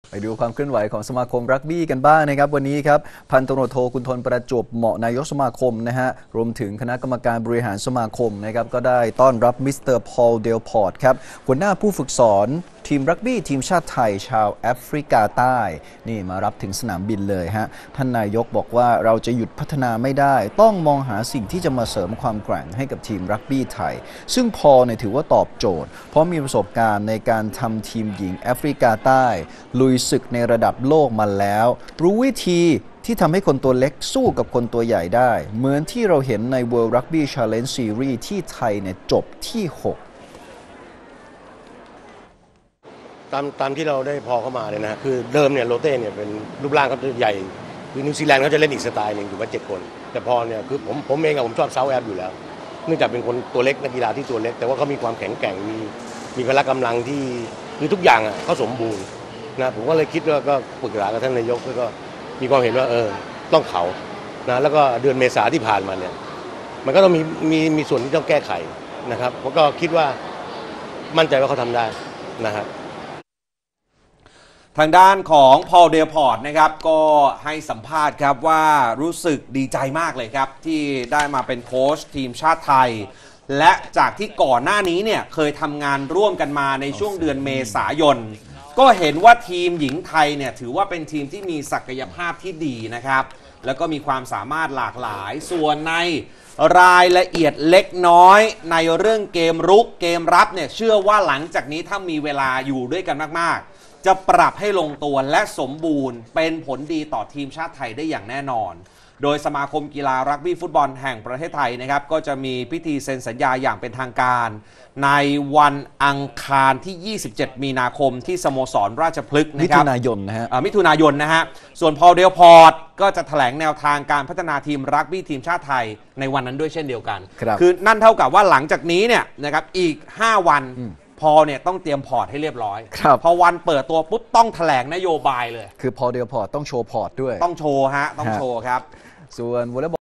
ข่าวกีฬา ต้อนรับ "พอล เดลพอร์ต" โค้ชรักบี้หญิงทีมชาติไทย ข่าวค่ำ วันที่ 22 มิถุนายน 2566
พ.ต.ท.กุลธน ประจวบเหมาะ นายกสมาคมกีฬารักบี้ฟุตบอลแห่งประเทศไทย นำคณะกรรมการบริหารสมาคม มาต้อนรับ พอล เดลพอร์ต ที่สนามบินสุวรรณภูมิ เมื่อวันที่ 22 มิ.ย.2566 โดย พ.ต.ท.กุลธน ประจวบเหมาะ เปิดเผยว่า ทีมชาติจะหยุดพัฒนาไม่ได้และต้องมองหาสิ่งที่จะเข้ามาเสริมความแข็งแกร่งให้ทีมรักบี้ไทย ซึ่ง พอล เดลพอร์ต ถือว่าตอบโจทย์ เพราะมีประสบการณ์ในการทำทีมหญิงทีมของแอฟริกาใต้ลุยศึกระดับโลกมาแล้ว อีกทั้งเชื่อว่าเจ้าตัวรู้วิธีที่ทำให้คนตัวเล็กสู้กับคนตัวใหญ่ได้ เหมือนอย่างที่เห็นกันใน เวิลด์รักบี้ชาเลนจ์ซีรีส์ ที่ทีมหญิงไทยได้อันดับ 6